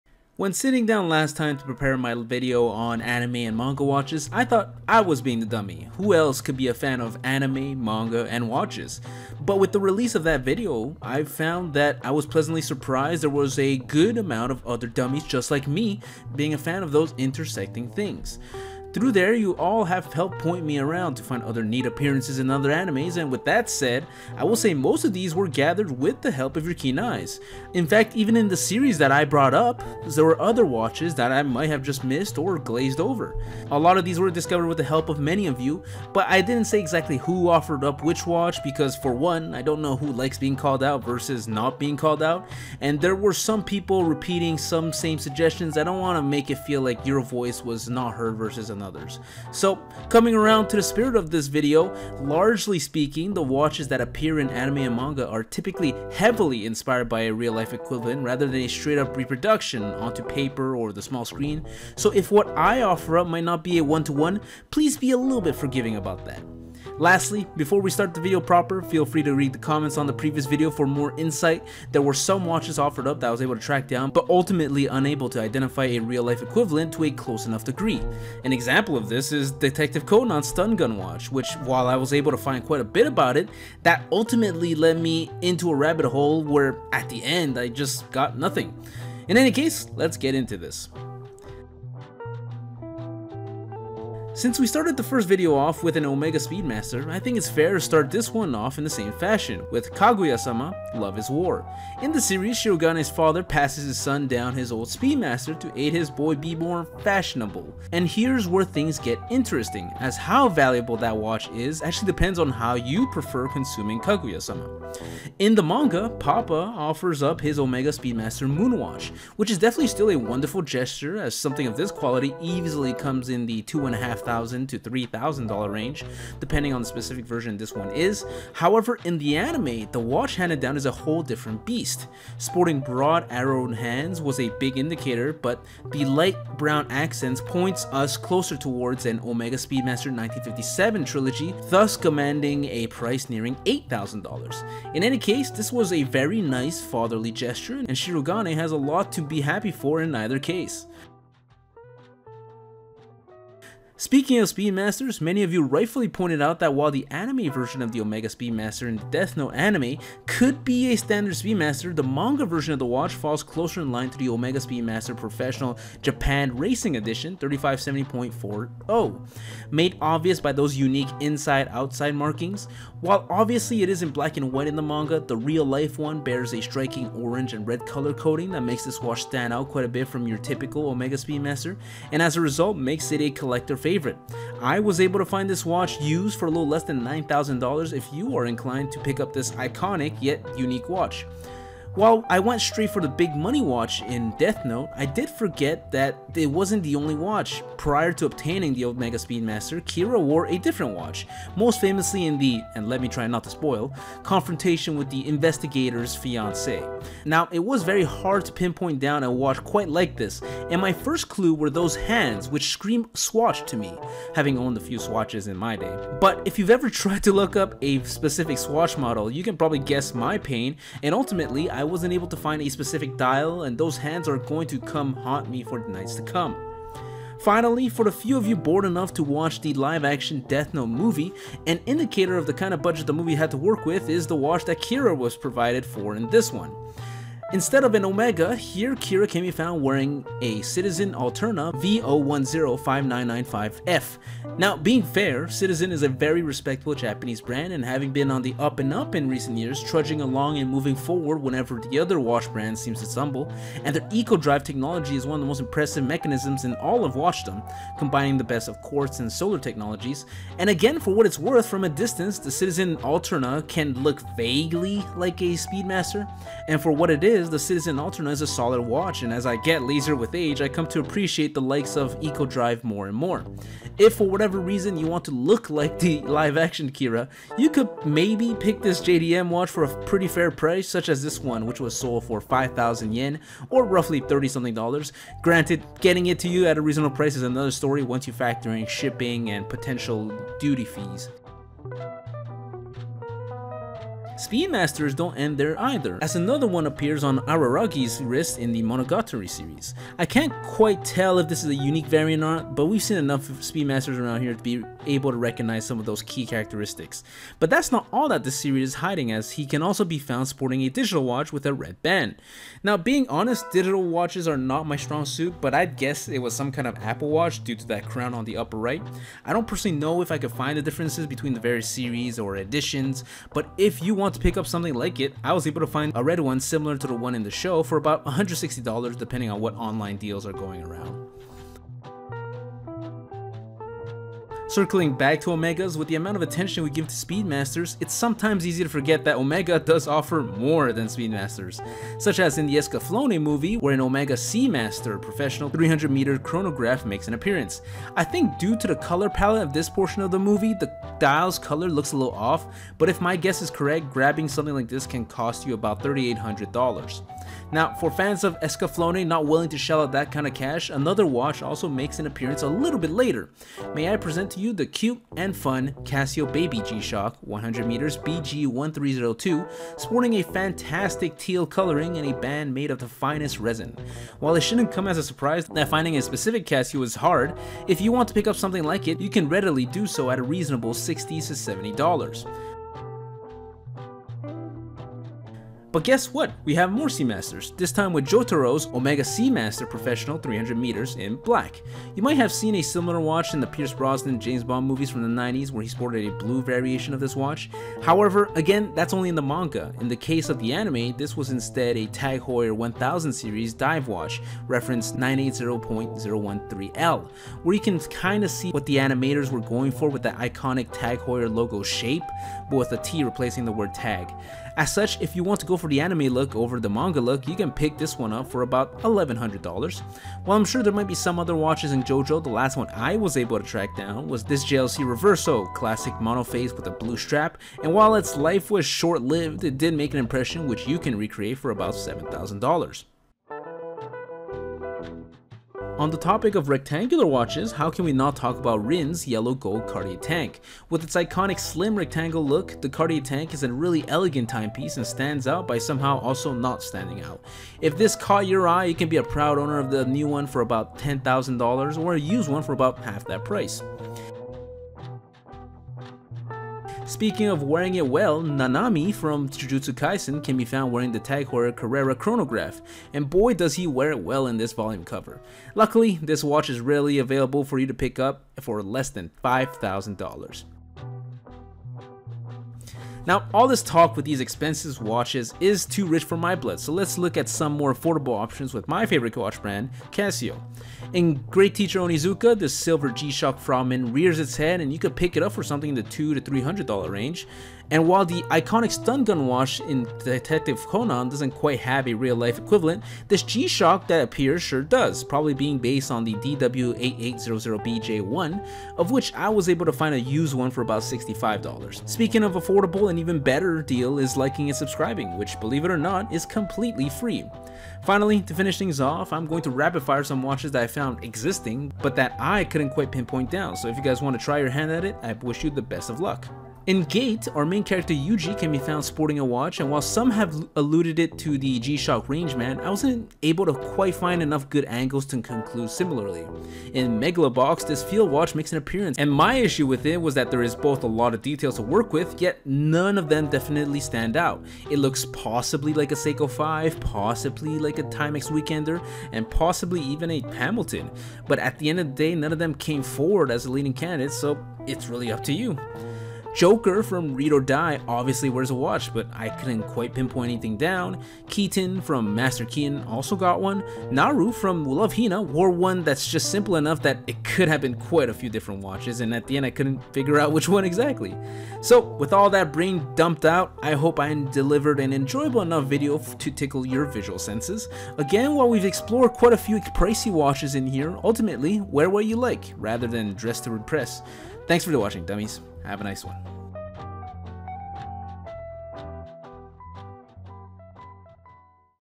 Let's Watch Anime For The Watches
With the good response from my last video I decided to make a continuation! Thanks a lot to all those that interacted and helped pinpoint some of these down.

ABOUT US:
This channel was created to give meaning to reading an unhealthy amount of manga.